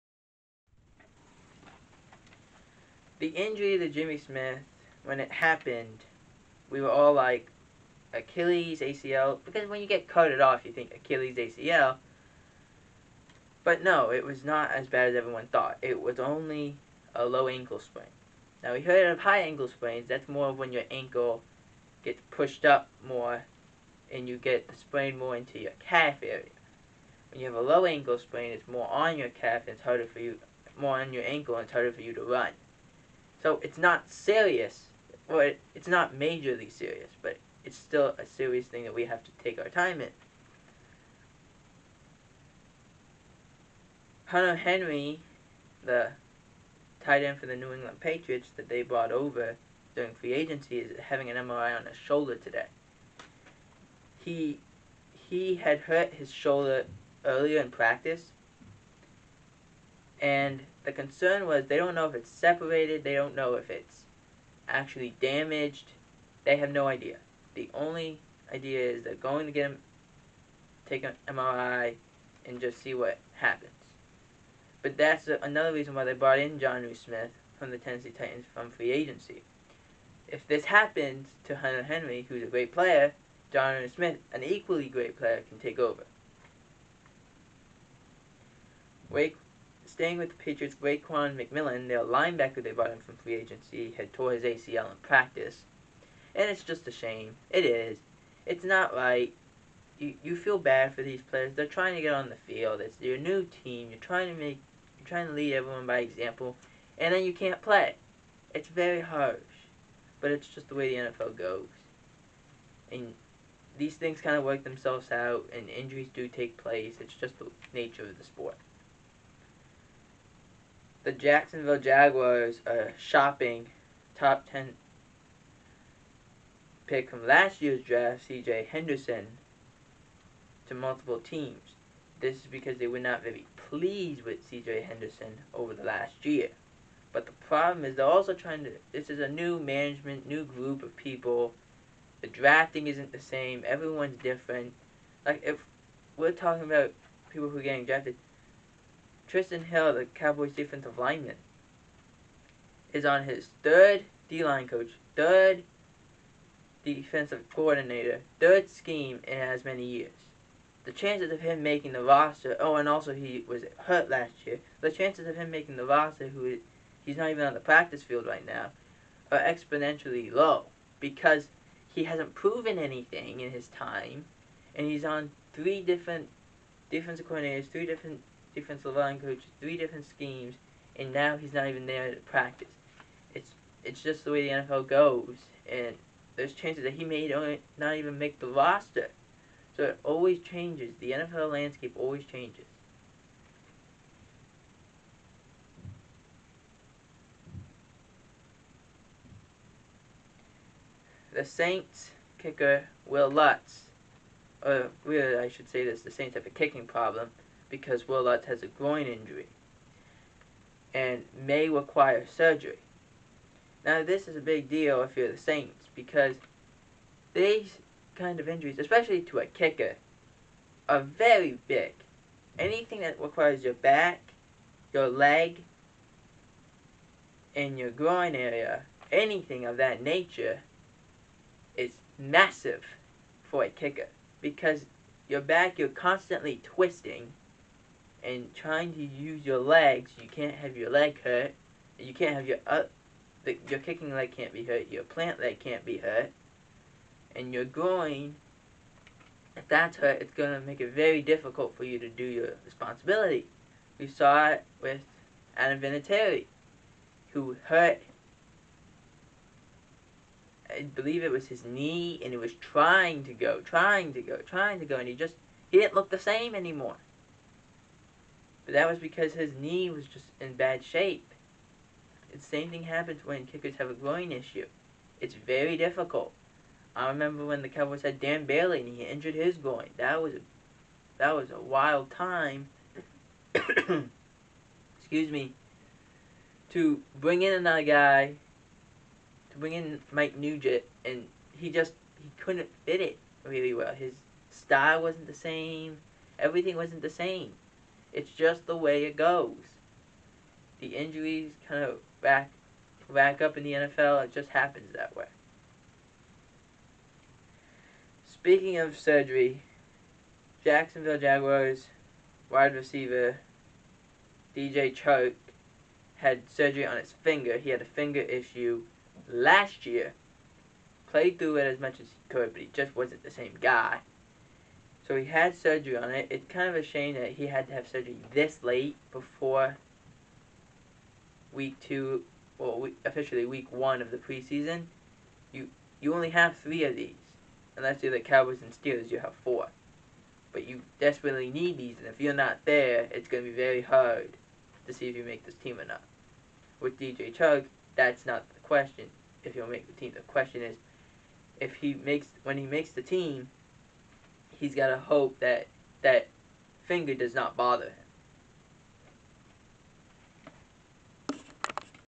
Jimmy Smith Dodges Serious injury, Jags Shopping top-10 pick from 2020 draft, Jags WR Surgery & more
Will Lutz injury.